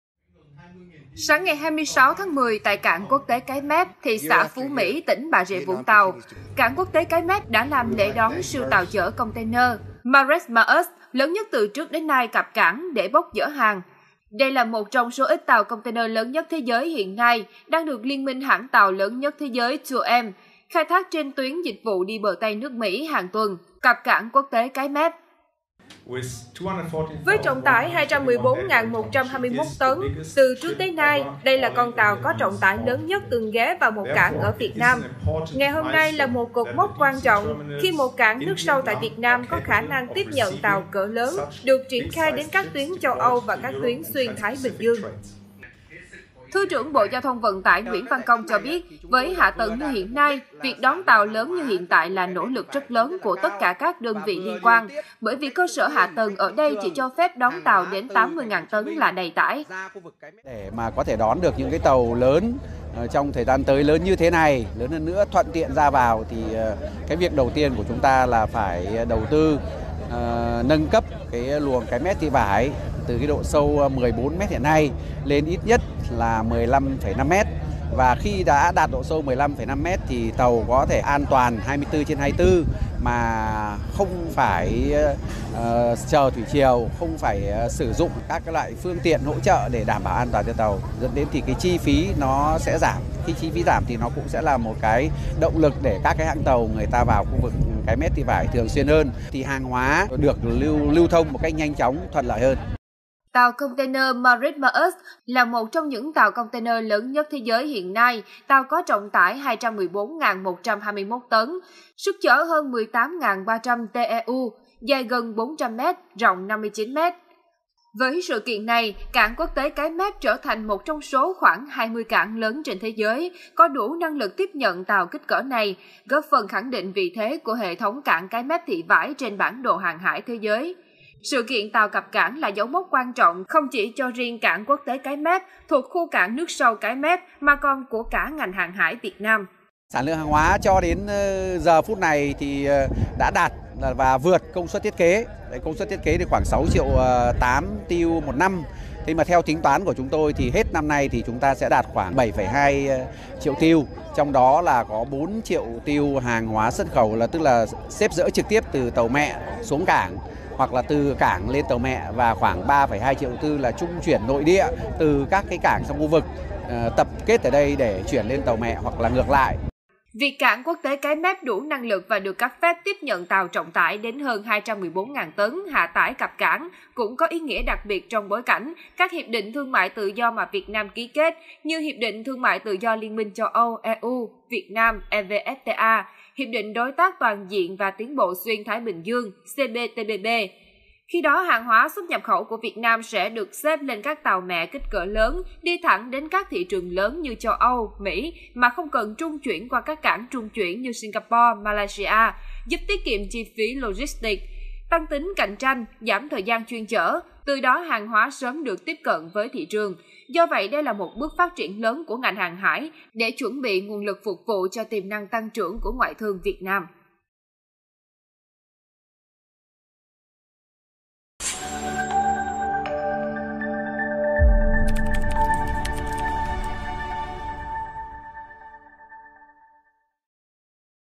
Siêu tàu container lớn nhất thế giới cập cảng Cái Mép
Ngày 26-10, tại Cảng Quốc tế Cái Mép (thị xã Phú Mỹ, tỉnh Bà Rịa - Vũng Tàu), CMIT là liên doanh giữa Tổng Công ty Hàng Hải Việt Nam và Cảng Sài Gòn và APM Terminals, đã làm lễ đón siêu tàu chở container Margrethe Maersk lớn nhất từ trước đến nay cập cảng để bốc dỡ hàng.